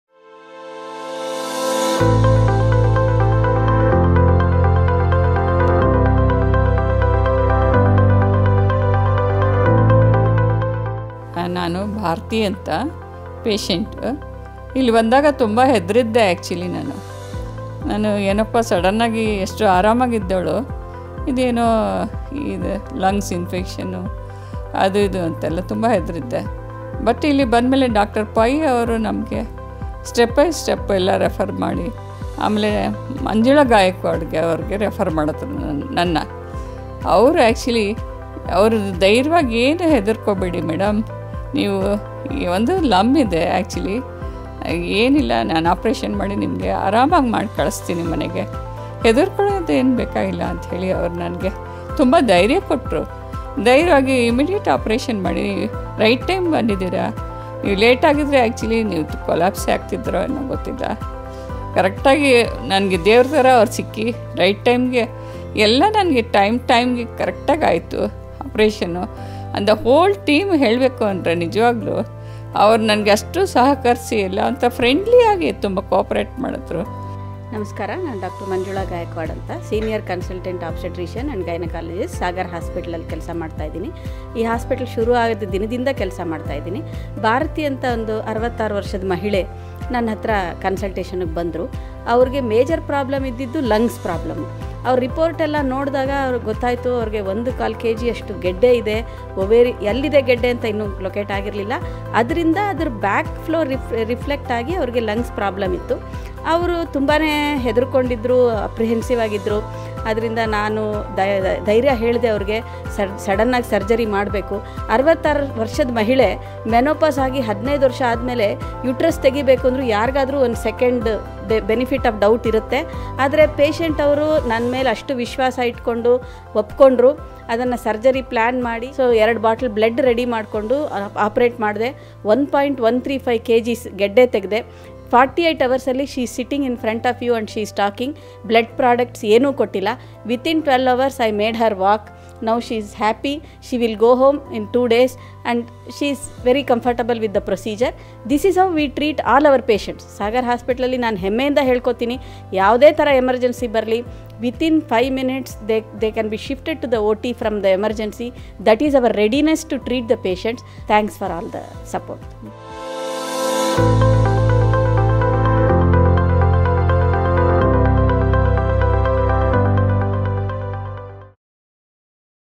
Happy Patient Happy Doctor
Mrs. Bharathi was referred to Dr. Manjuala Gaekwad M, Senior Consultant Obstetrician & Gynecologist for consultation. It was revealed to he surprise that she had fibroid growing in her uterus which had to be operated. the operation was successful. She has appreciated the team for their friendly nature and cooperation towards her treatment. Dr. Manjula also shares her experience while treating Mrs. Bharathi & is happy with her recovery.